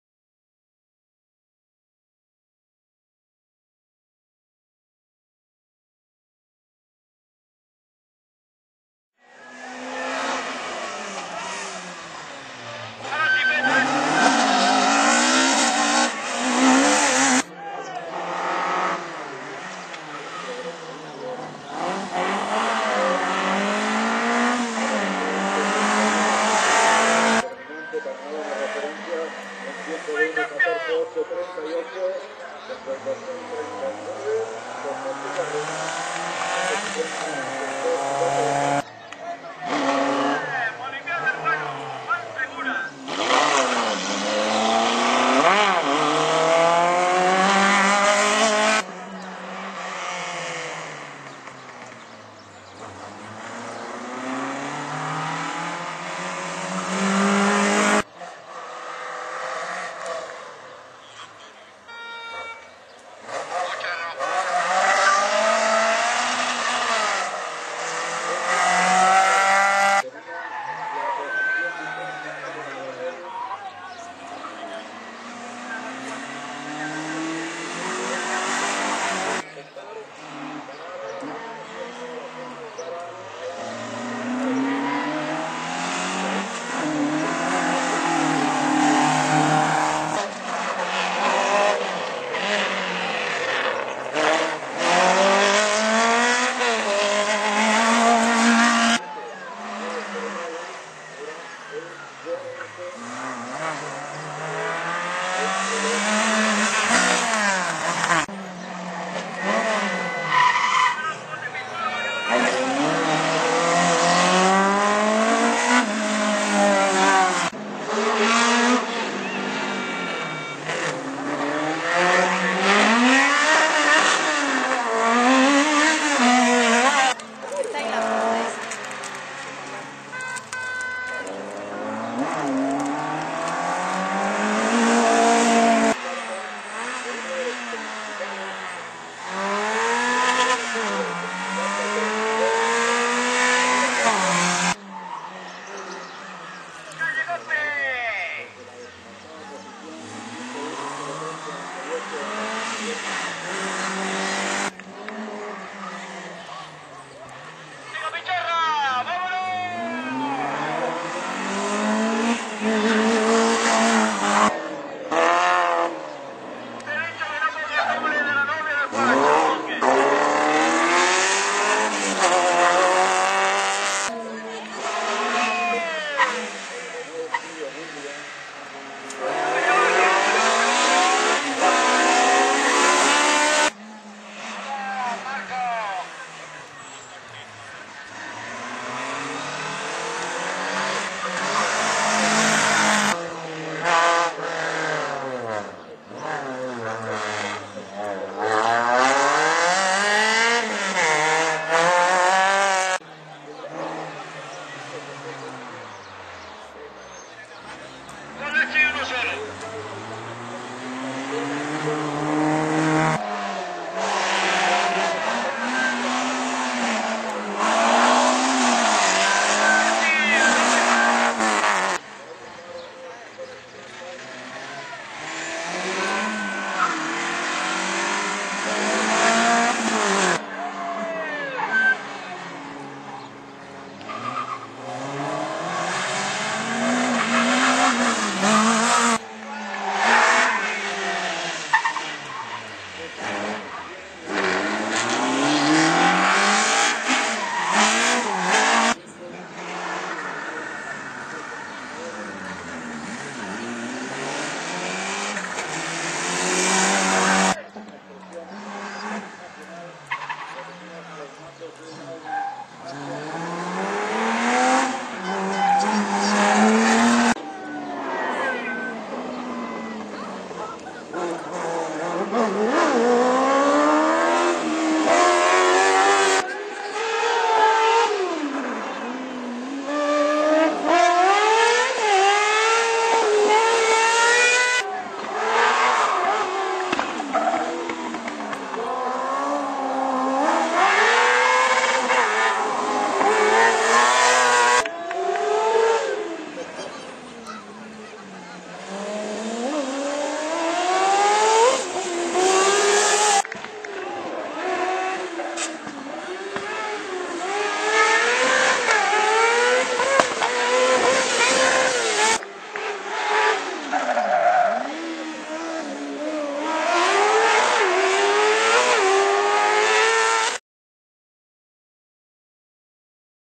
I Cronometrada de Níjar (Almería)
Primera Manga oficial. Paso de todos los participantes.